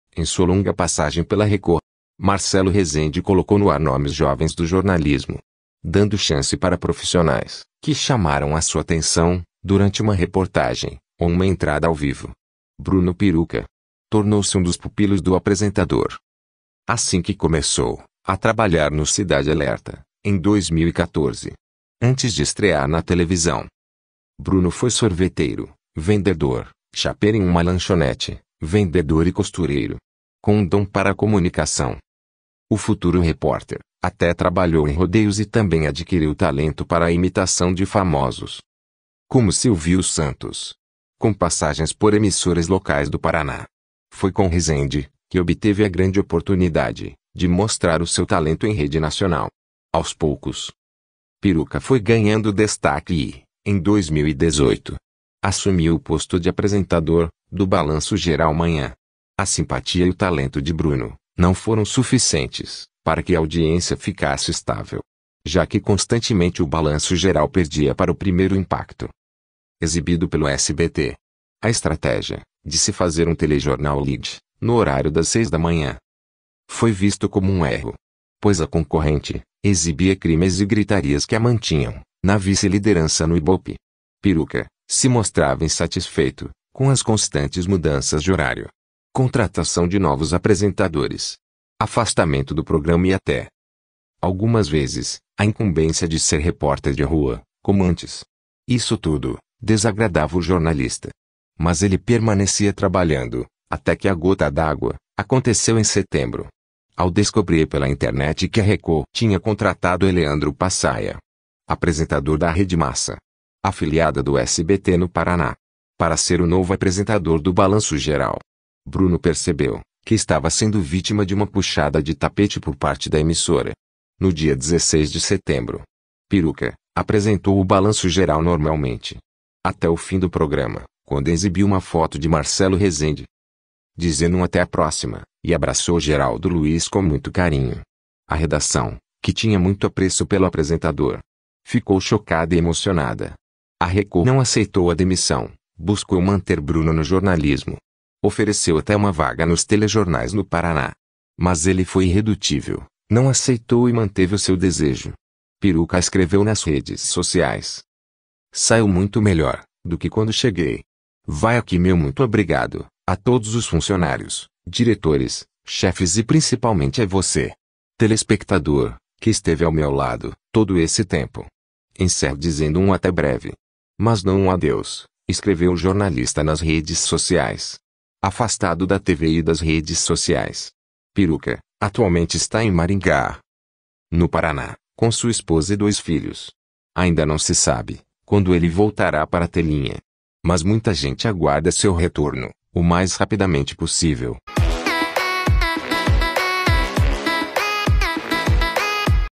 Puxada de tapete: se demitiu da Record, entenda O que aconteceu
Notícias, informações, noticiários, entretenimento, humor, curiosidades, mantenha-se informados.
Bruno Peruka.
 tornou-se um dos pupilos do apresentador.
assim que começou, a trabalhar no Cidade Alerta,
em 2014.

Antes de estrear na televisão.
Bruno foi sorveteiro, vendedor, chapeiro em uma lanchonete, vendedor e costureiro.

Com um dom para a comunicação.
o futuro repórter, até trabalhou em rodeios e também adquiriu talento para a imitação de famosos.
como Silvio Santos.